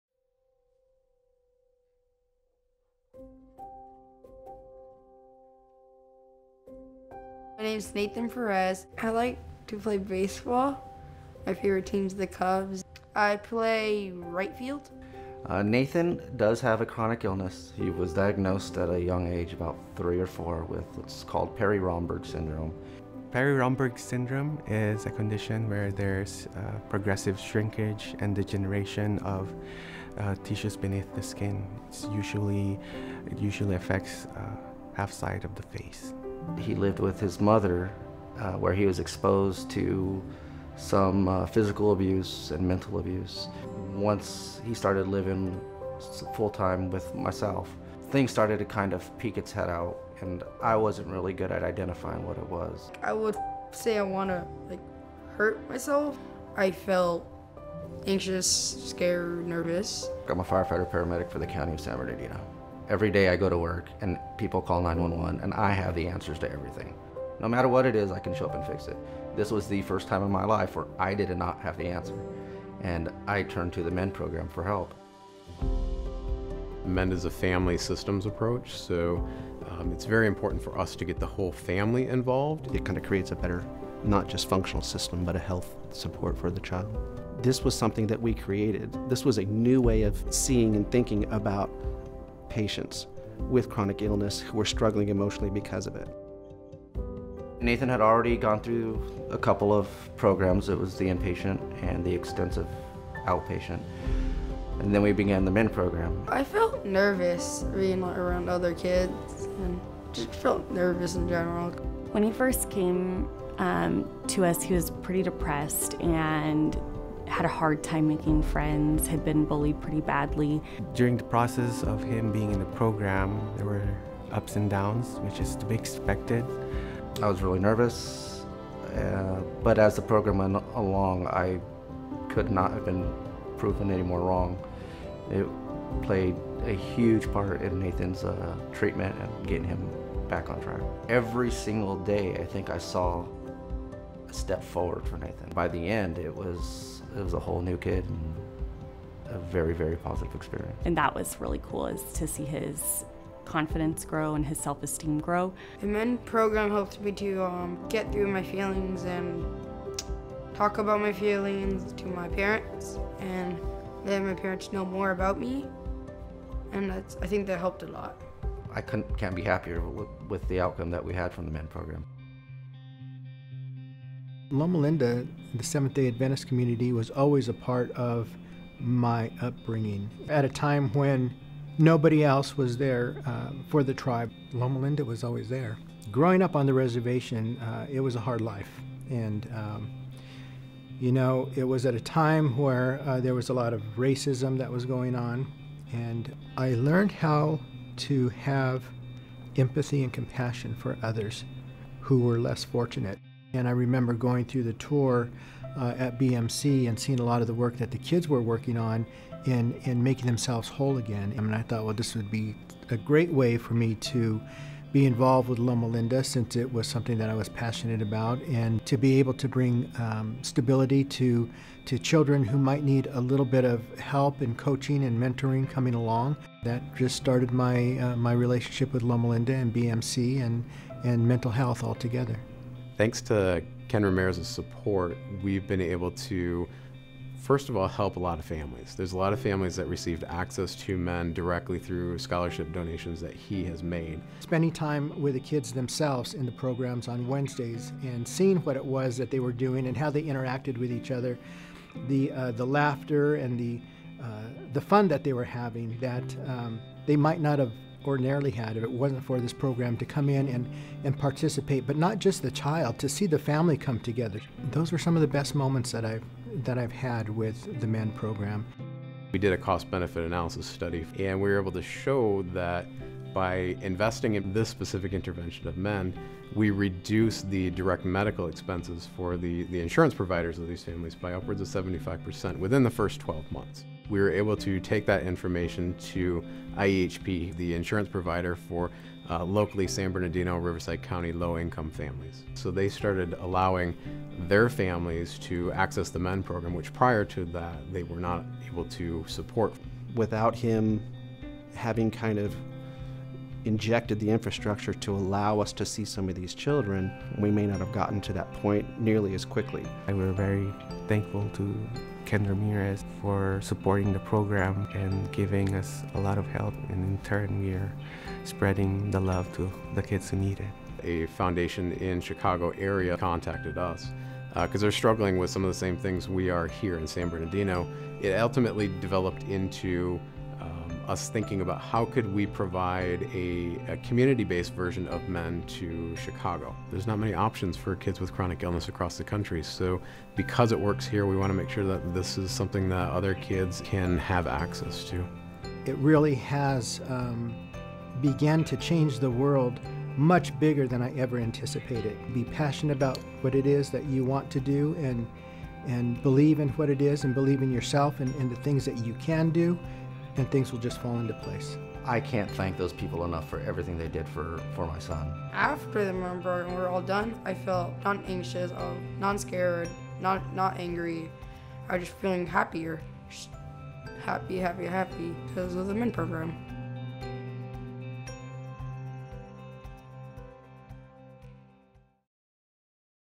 Investing in Compassion - Seeds of Hope
The MEND program was created at Loma Linda University Health to support the patients and their families in maintaining or regaining emotional health and balance during the process of a significant medical illness or treatment such as transplantation, diabetes management, or chemotherapy.
Thanks to generous partners like Ken Ramirez, this program has been able to develop, strengthen, and expand.